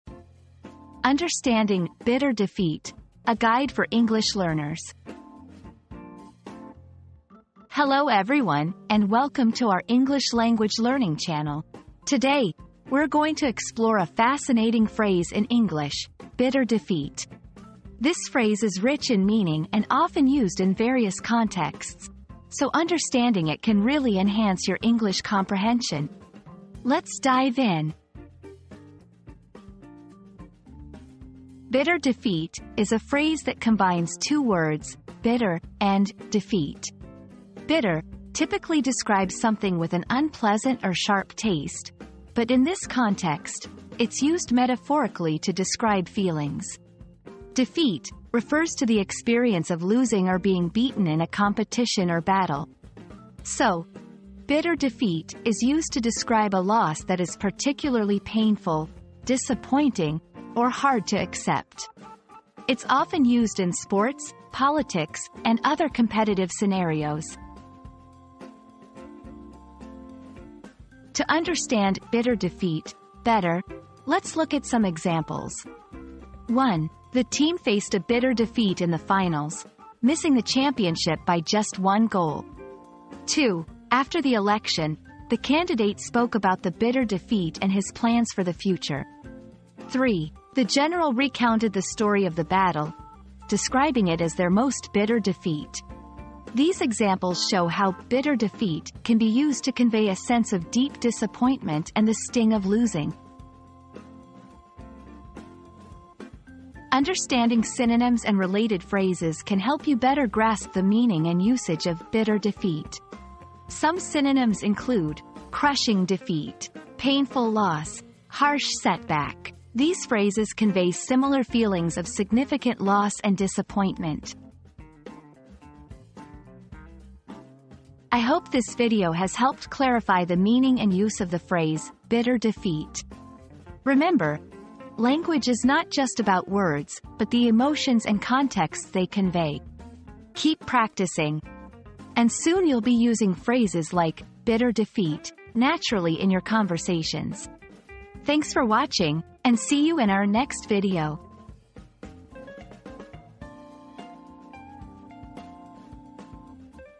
Understanding "Bitter Defeat": A Guide for English Learners
00:00 • Introduction - Understanding "Bitter Defeat": A Guide for English Learners
00:31 • What Does "Bitter Defeat" Mean?
01:10 • Examples of "Bitter Defeat" in Sentences
01:46 • Synonyms and Related Phrases